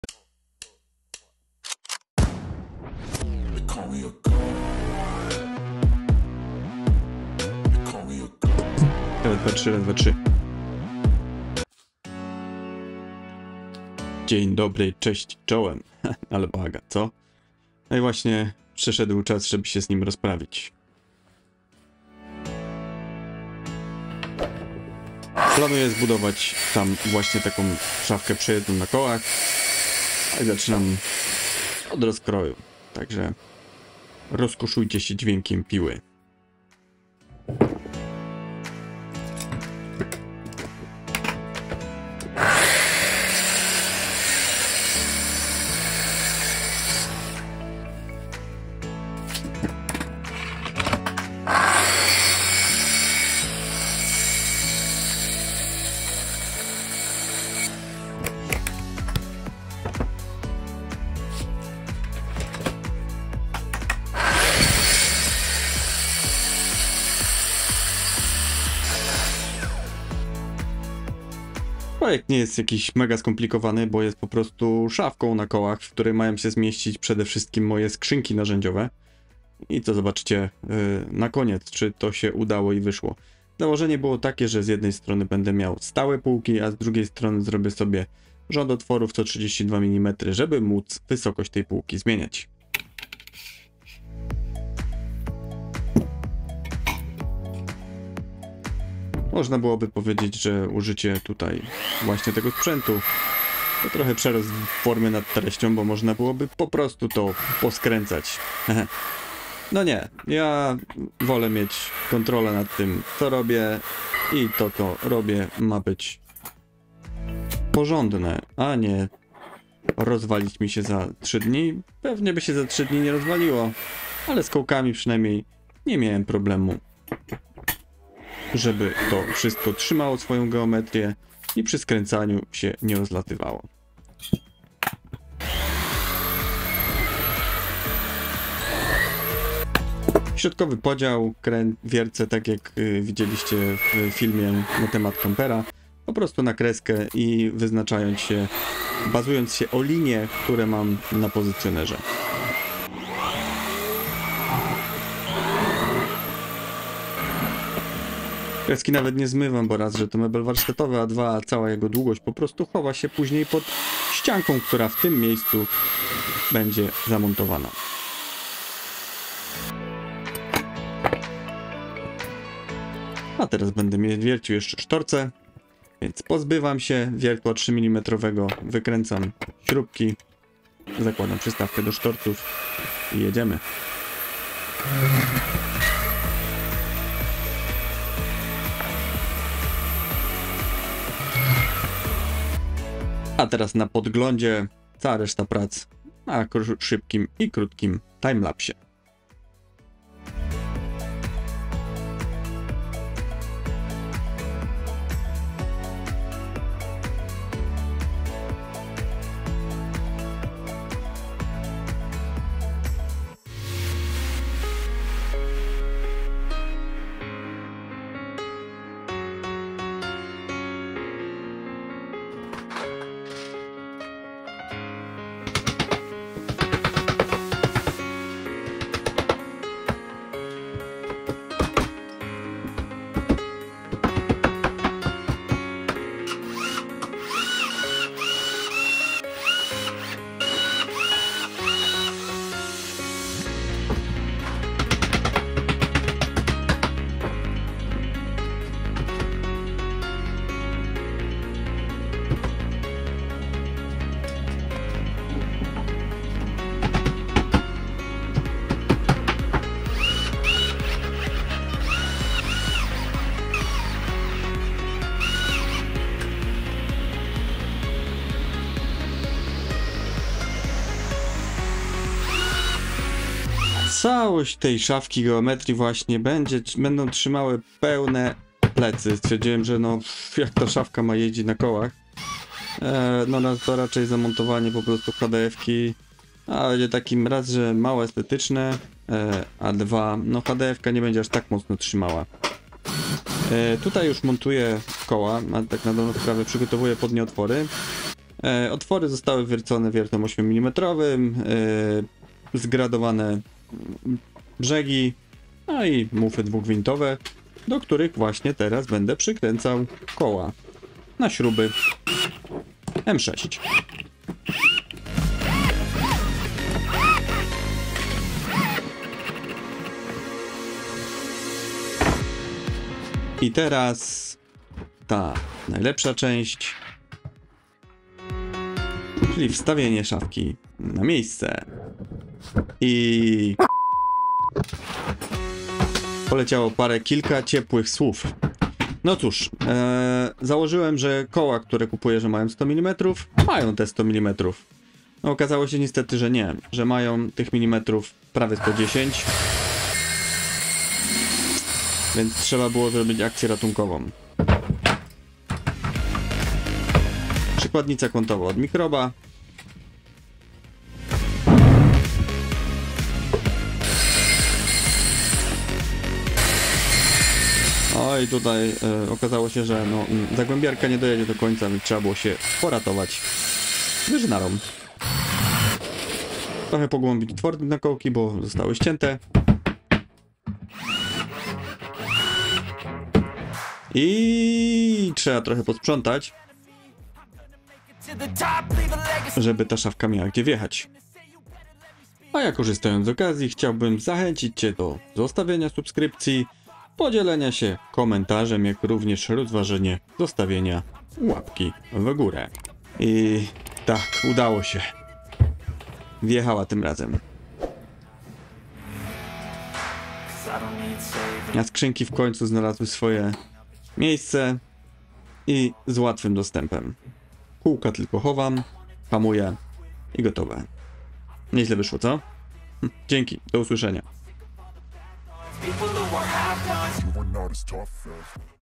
Warsztatowa szafka na skrzynie narzędziowe na kołach | Ale wtopa....
Cześć,

Dzisiaj mały projekt warsztatowy. Zabrałem się za małe porządki w moim warsztacie i zacząłem od kąta pod warsztatem.

Jedna część dopasowana na moje LBoxy, druga z rzędem otworów w systemie 32, bo nie wiem co tam się jeszcze znajdzie.

Trochę dźwięków warsztatu, zagłębiarki wiertarki dwuwrzecionowej.